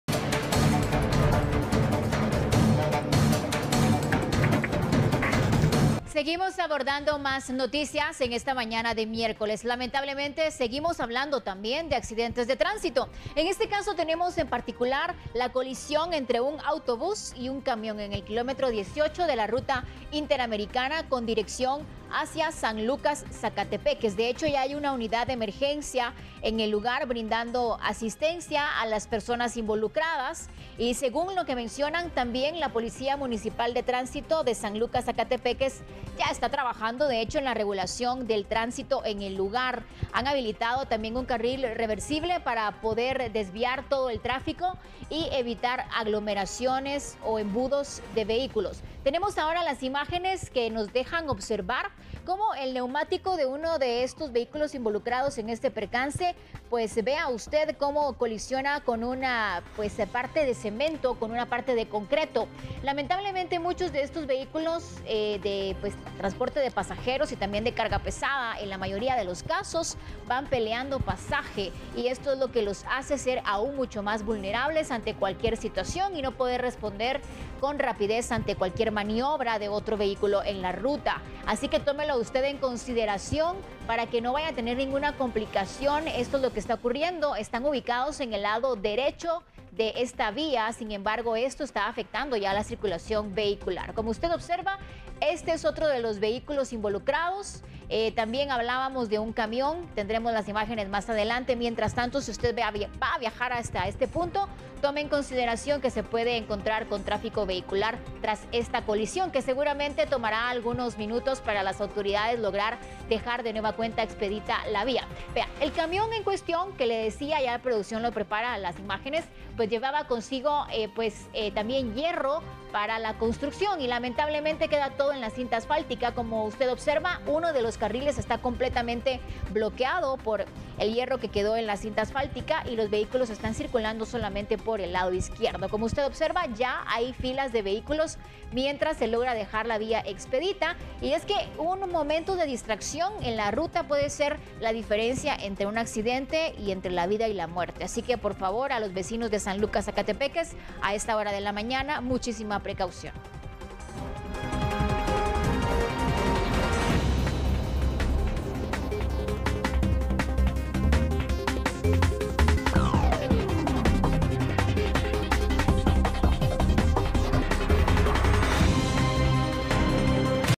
Colisión entre bus y camión se registra en Ruta Interamericana
Un accidente de tránsito se registró en el kilómetro 18 de la Ruta Interamericana, donde un bus y un camión colisionaron, dejando un carril obstaculizado y está afectando la circulación vehicular.